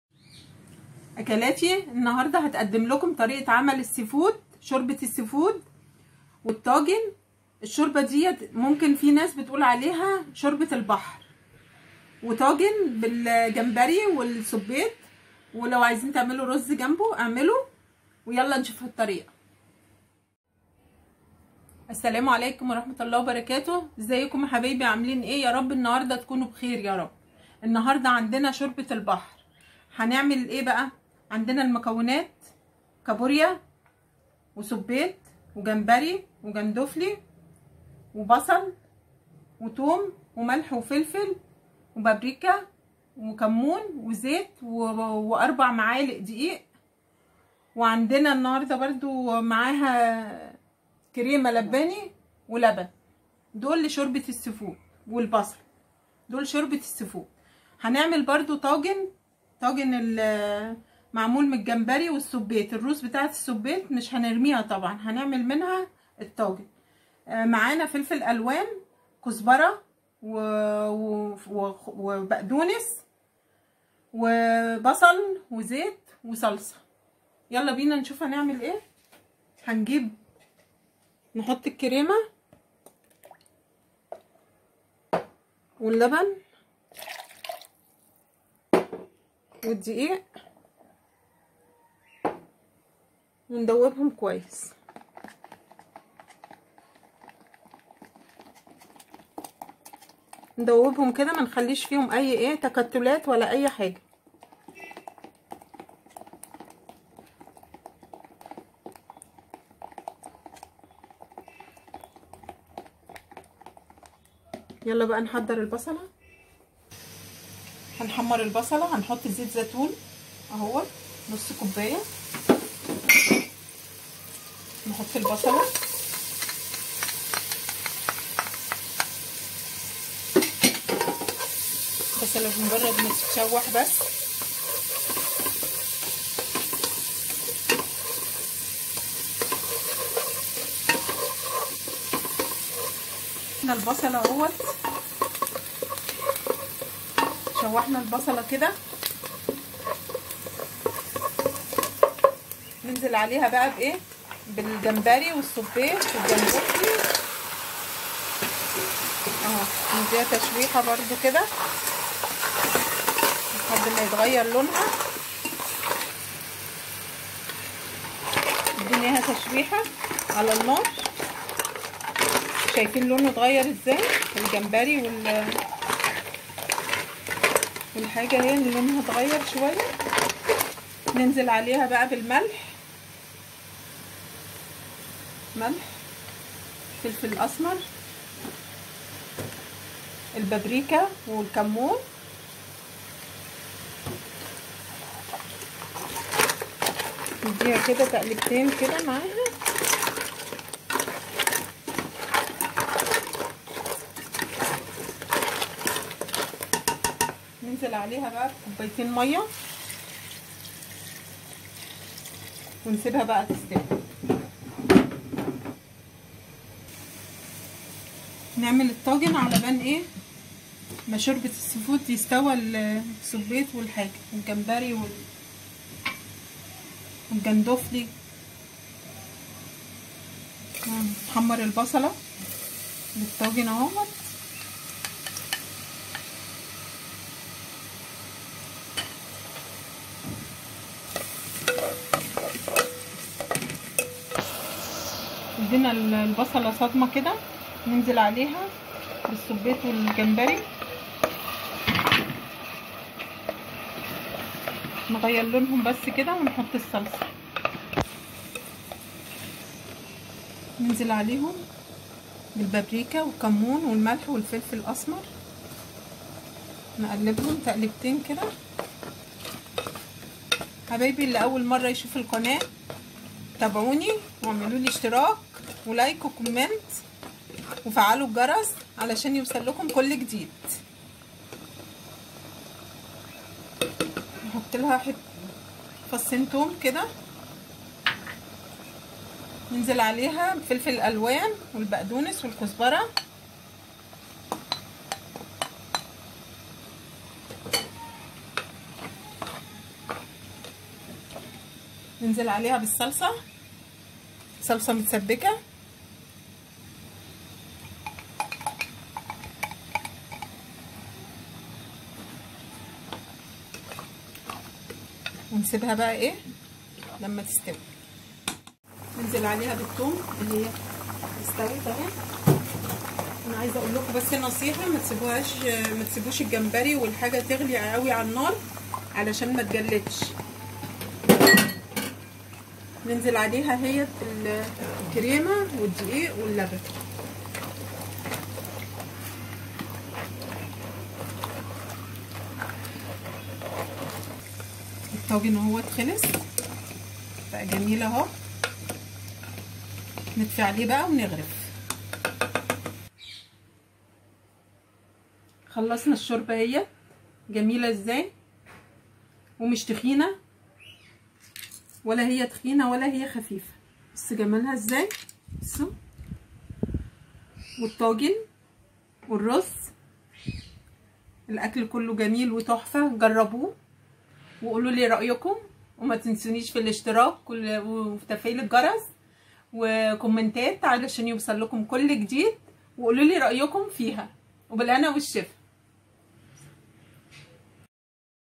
يالا تعالي معانا شوفي طريقه السي فود
كابوريا 🦀

جمبري 🦐

سبيط 🦑

جندوفلي

بصل

زيت زيتون

ملح

فلفل اسود

بابريكا

لبن

كريمه لباني

4 م دقيق

فلفل الوان

بقدونس

كرفس

ثوم

كوب صلصه